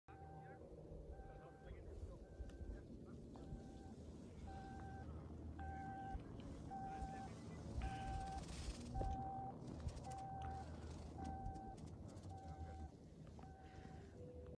【FWT】LOTTE ARAI FJT FWQのコースを上から撮影 〜大会の参考に〜
2022 Freddie Junior(Qualifier) Tourのコースの、マムシコースを本番当日に上から撮影したものです。
大会に出場される方の参考になれば幸いです。